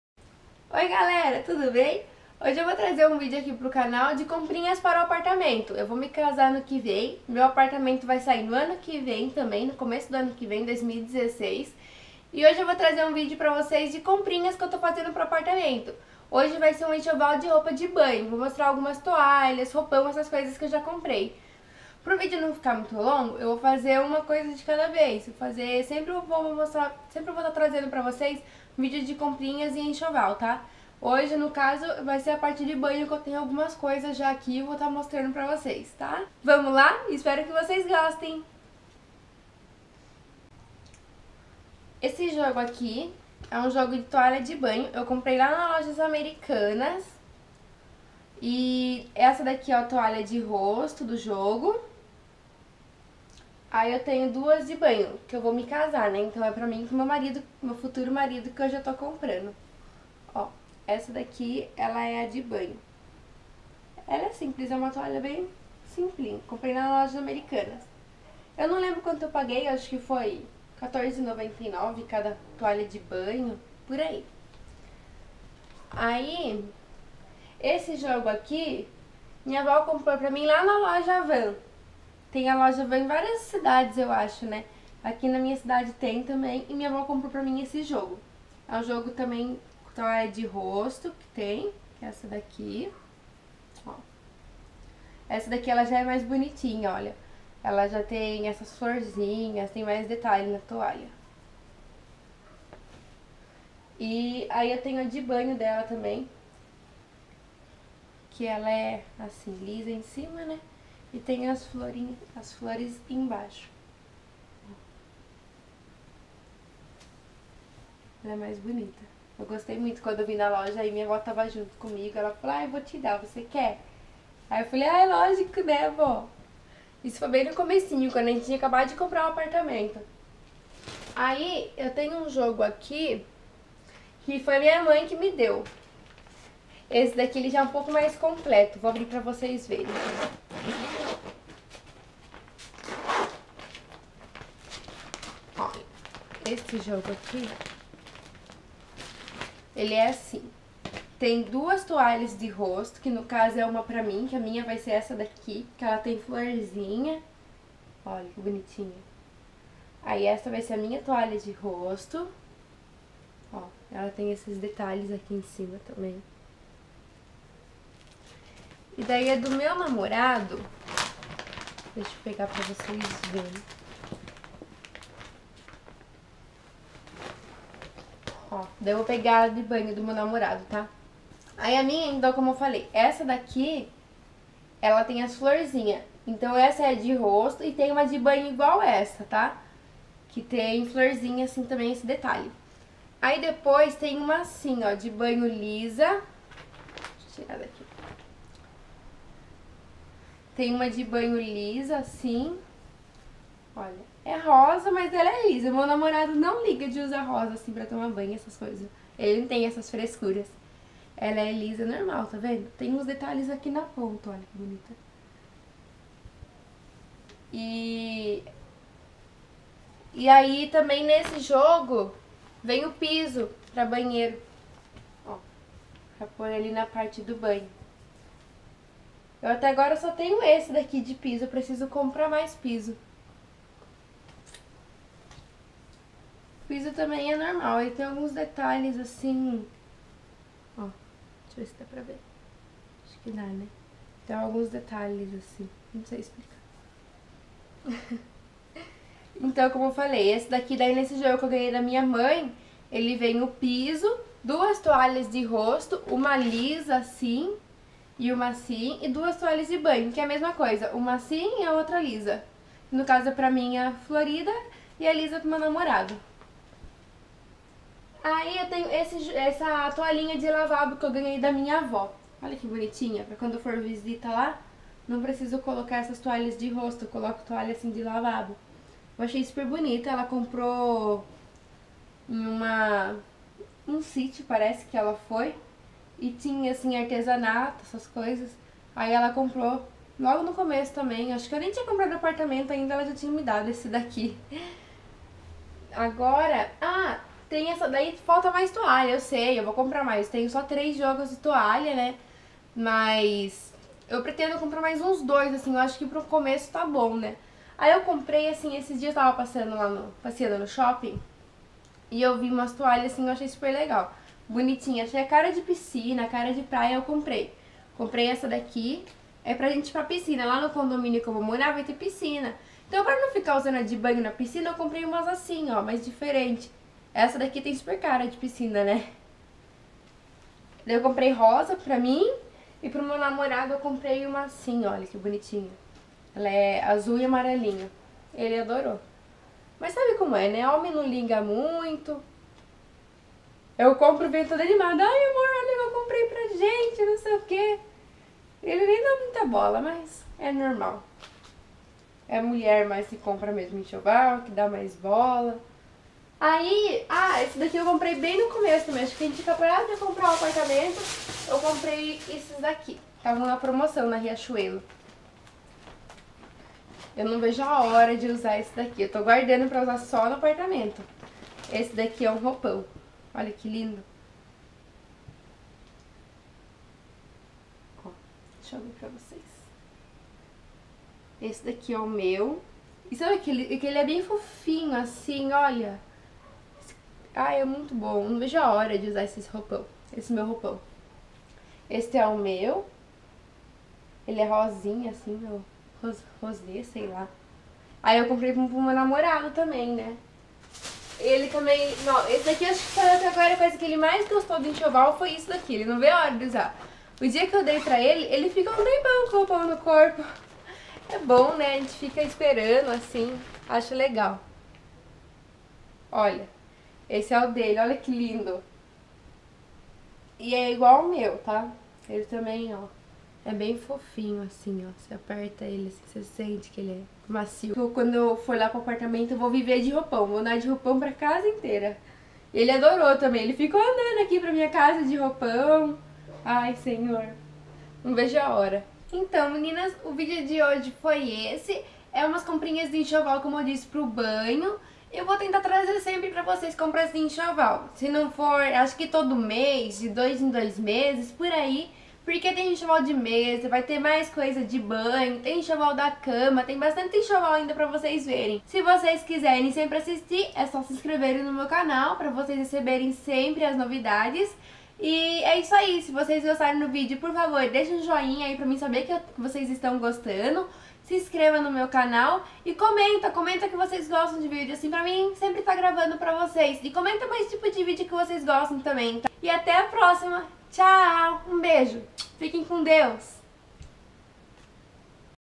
Comprinhas para o Apê - Meu Enxoval Banho
Olá galera, tudo bem?! Hoje trouxe um pouco do meu enxoval do banho para vocês, espero que gostem.

Beijos, fiquem com Deus!